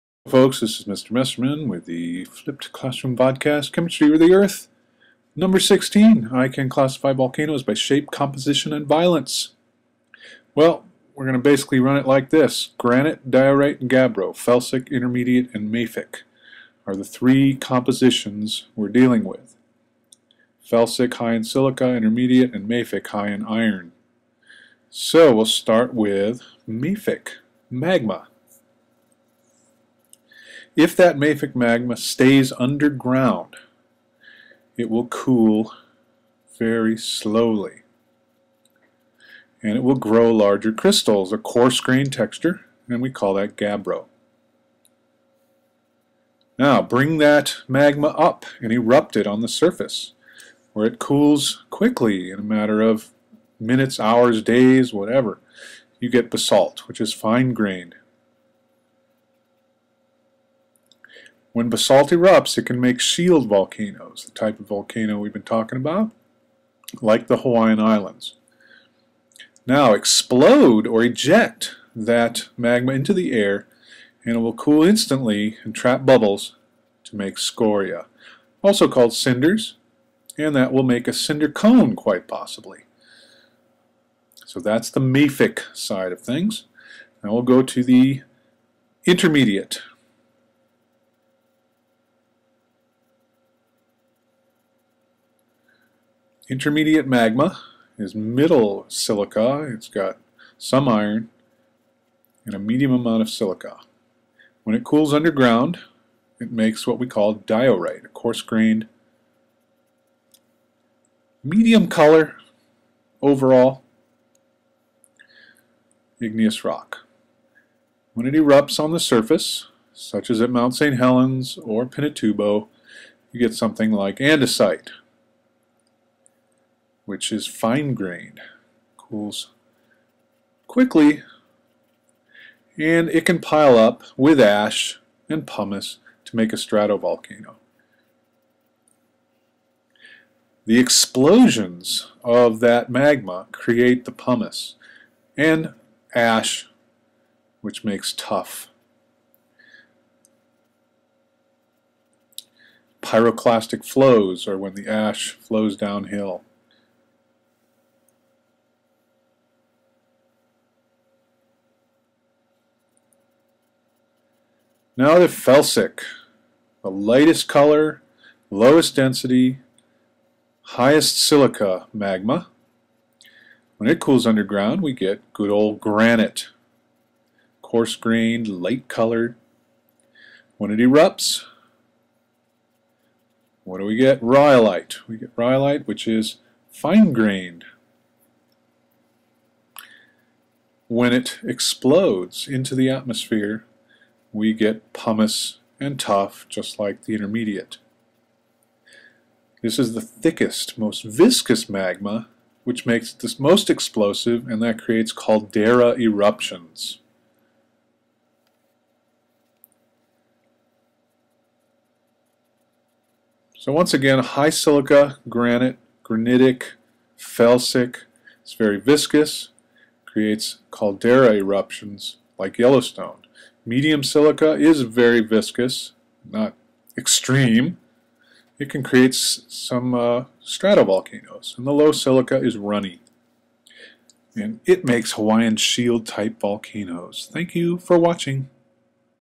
COE16 Volcanoes
Overview of how mafic, intermediate, and felsic magmas will create intrusive and extrusive igneous rock and volcanic structures.